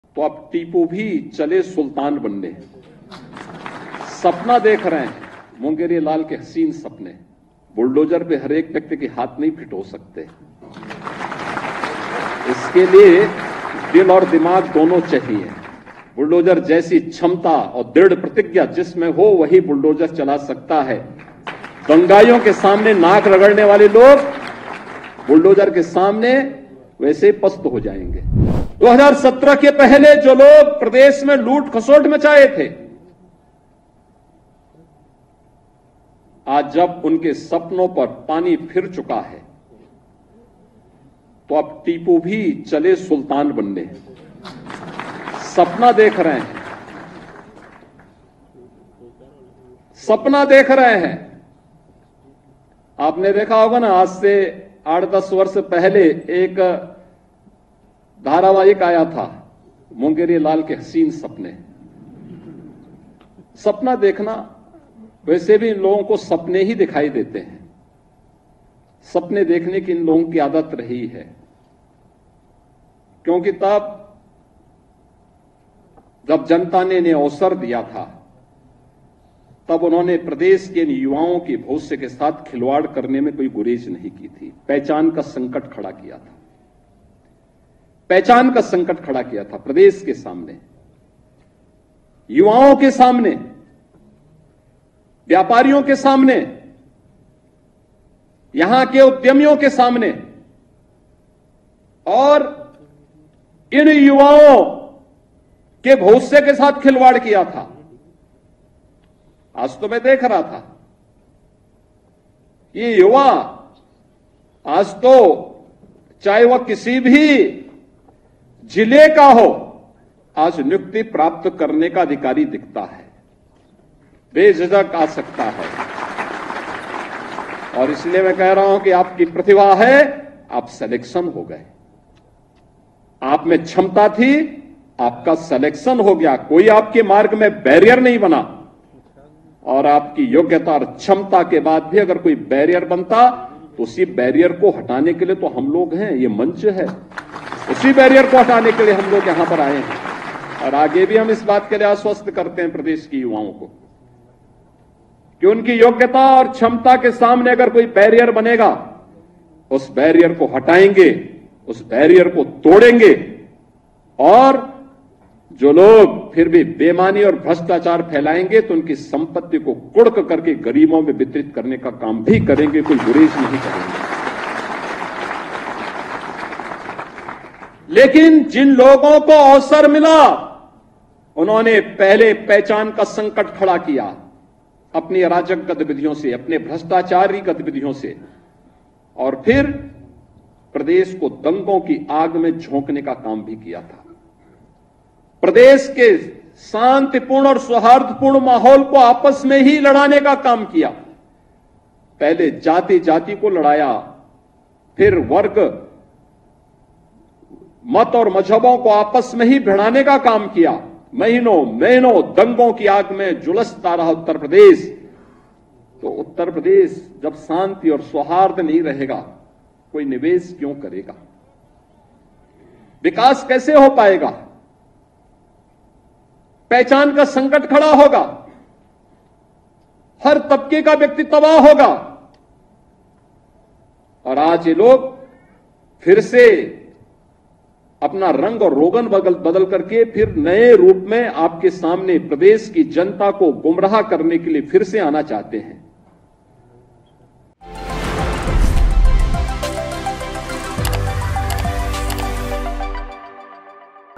Bulldozer Action पर CM Yogi का करारा जवाब, Akhilesh Yadav के बयान पर कसा तंज | Supreme Court | NBT
सपा प्रमुख अखिलेश यादव का सरकार बनने के बाद बुलडोजर एक्शन को लेकर दिए गए बयान पर सीएम योगी का करारा जवाब सामने आया है । सपा के मीडिया सेल की तरफ से जारी किए गए प्रेस रिलीज में कहा गया था कि  सरकार बनते ही पूरे प्रदेश के बुलडोजरों का रूख गोरखपुर की तरफ होगा । जिस पर सीएम योगी ने पलटवार करते हुए कहा कि बुलडोजर चलाने के लिए हिम्मत चाहिए ।